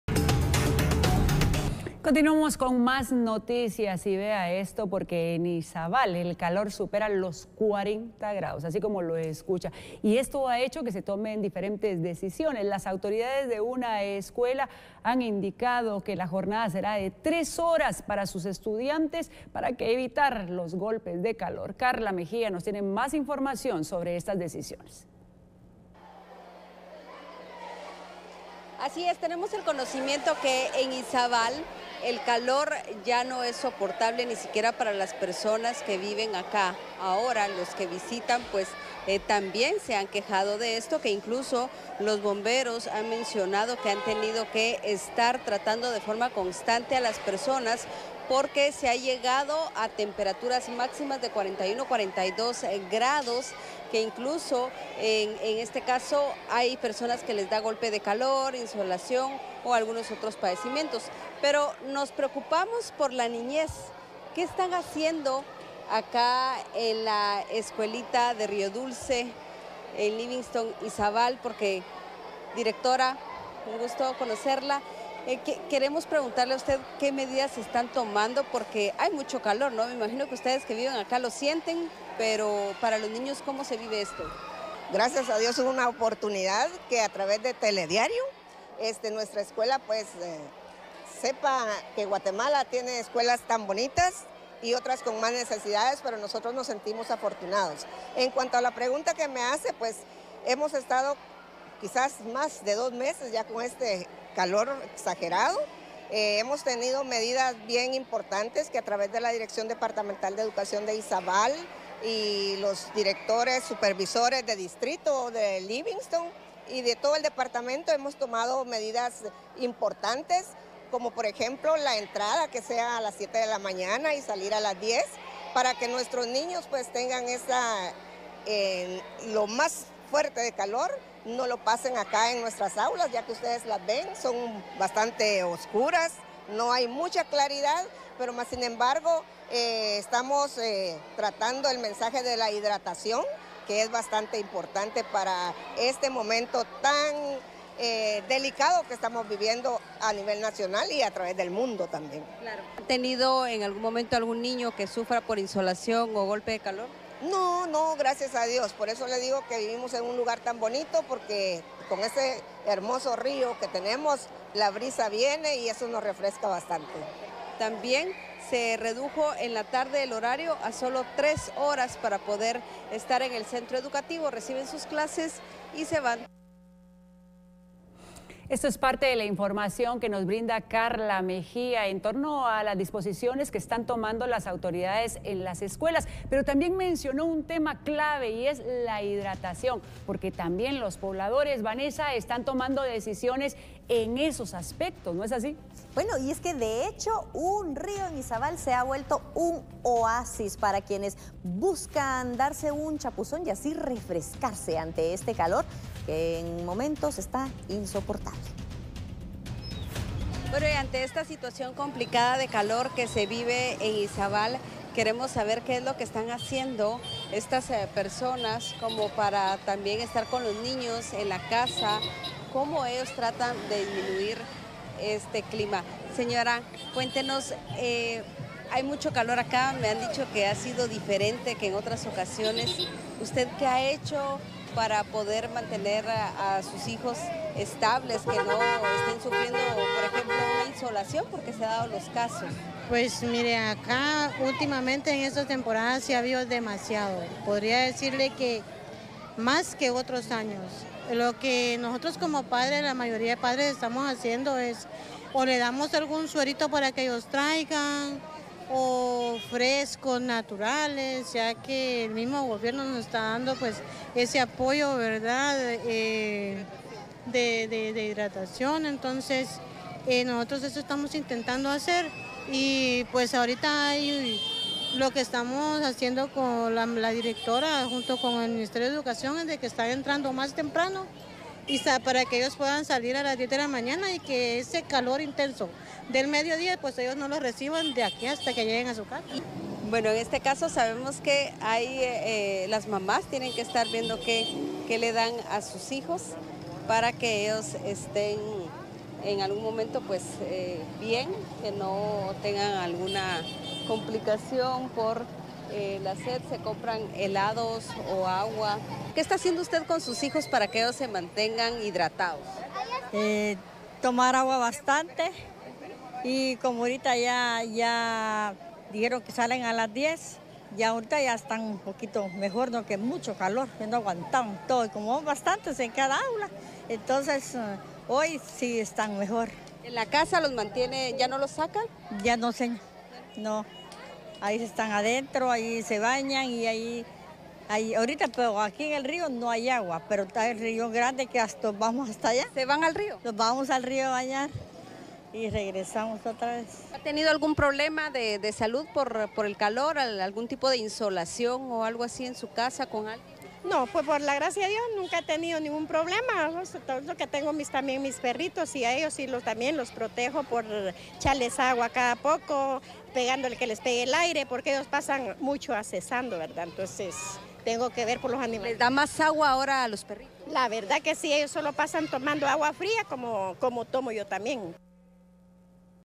Reducen horario de clases por olas de calor
Autoridades educativas tuvieron que tomar medidas de prevención para evitar que los menores sean expuestos a las altas temperaturas.
El calor en Izabal ha llegado a superar los 40° C, siendo necesaria la intervención de los Bomberos por los golpes de calor.